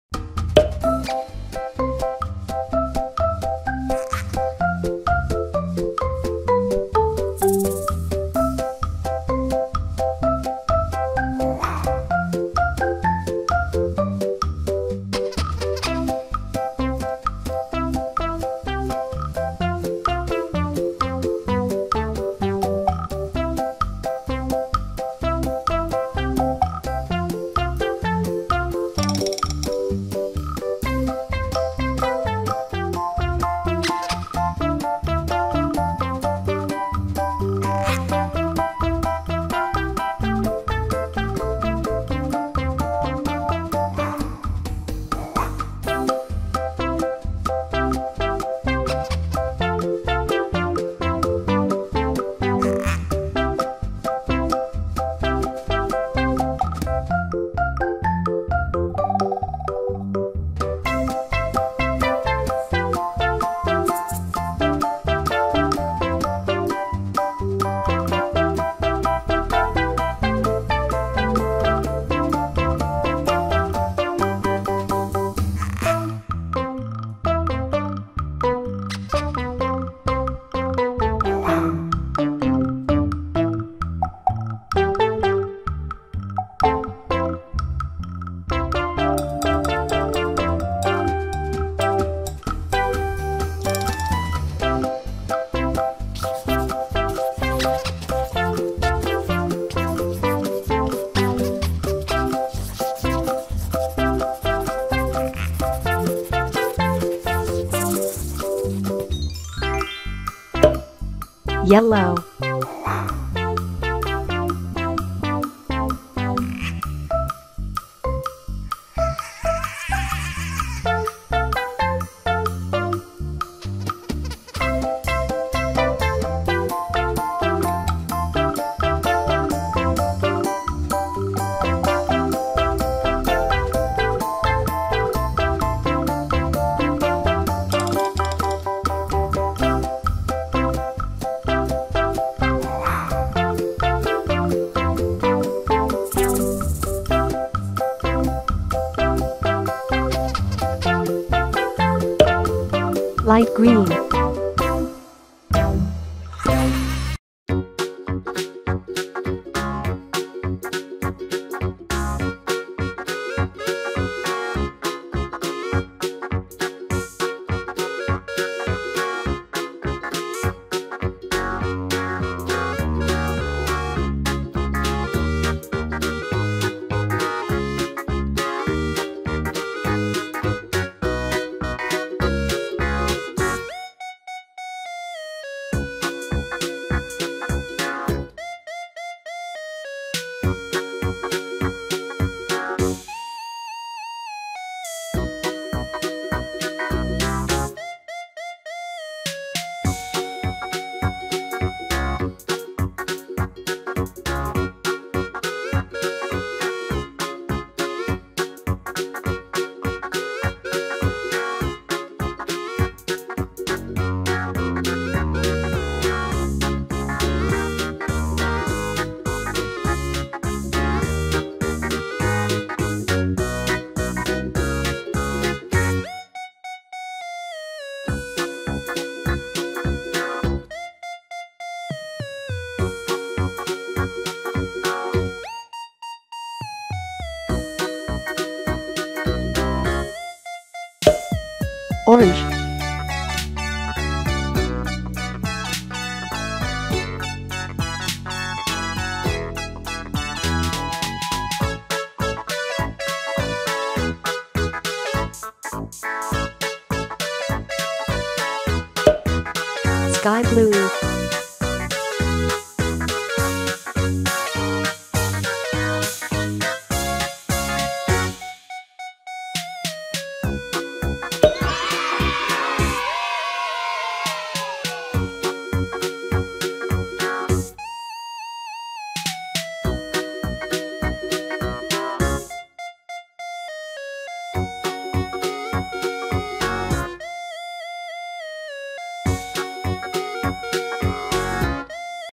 Menggambar Dan Mewarnai katak - Menggambar dan mewarnai Mainan binatang Untuk Anak-anak
Saya akan mengajar mengajarkan Anda cara menggambar dan mewarnai Berang-Berang Mainan (toy forg) Dan bantu belajar warna untuk anak-anak